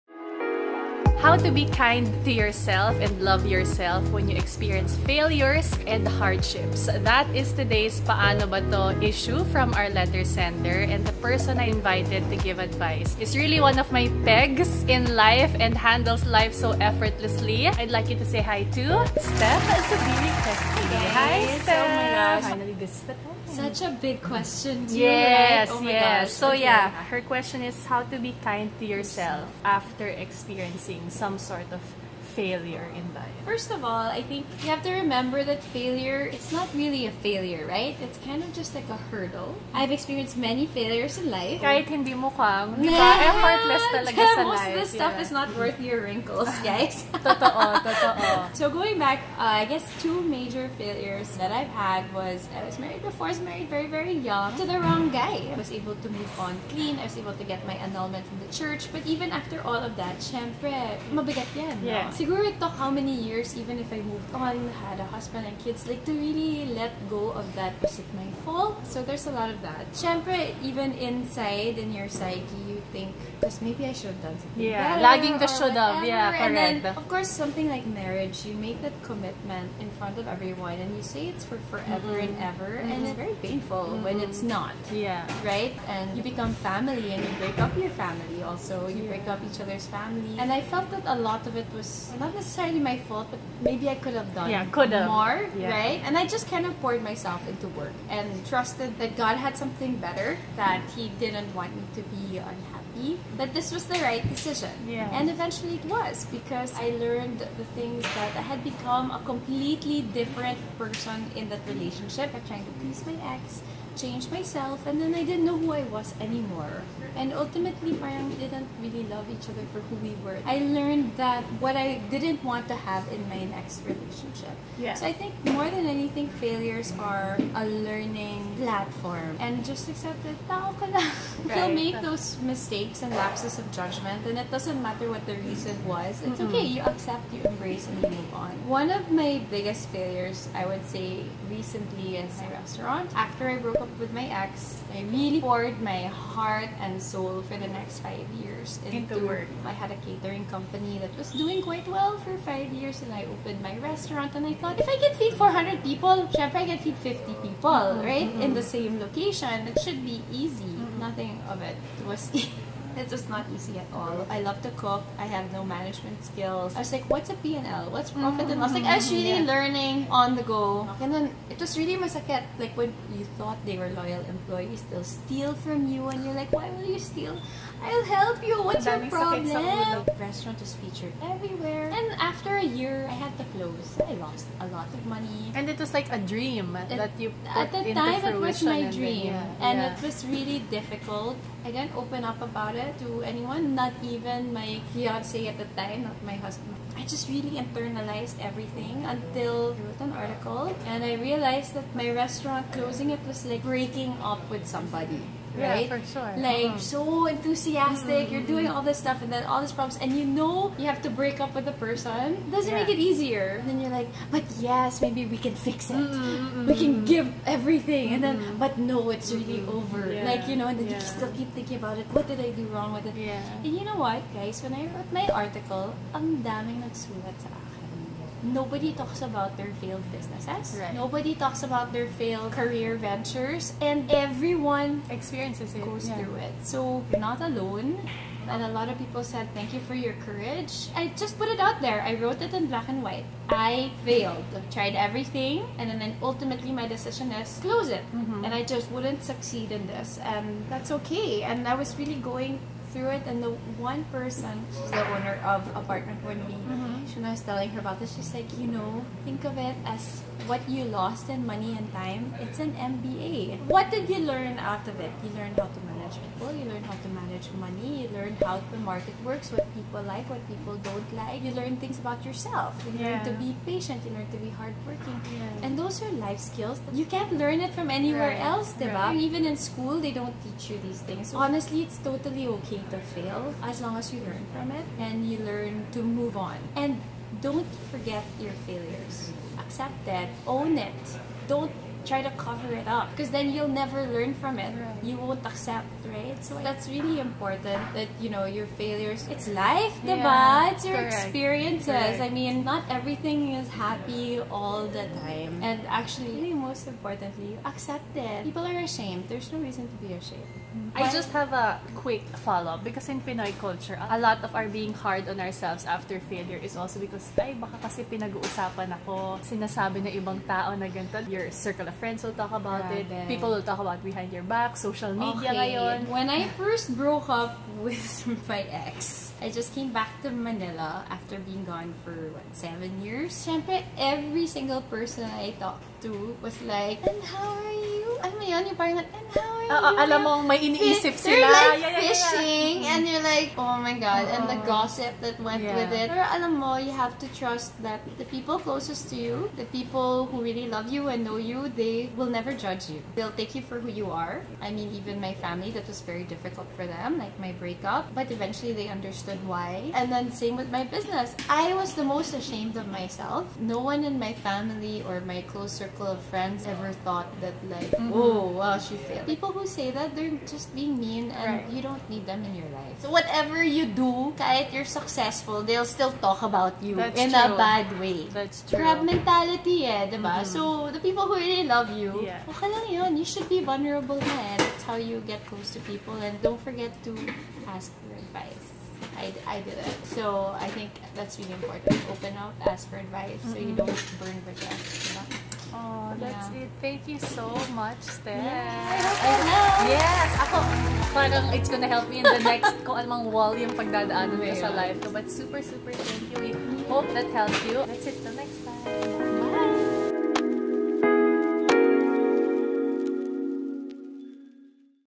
"How To Deal With Failure?" | Paano Ba 'To with Stephanie Zubiri
How to be kind to yourself after experiencing failures and hardships? This LOVE month, we continue our series on self love. How do you still love yourself after feeling you've failed? Real life superwoman Stephanie Zubiri shares her very personal experience and what she has learned.
To read Steph's article on failure that she mentioned in the video, click this
Produced and shot by me
Edited by Kirstin Alonzo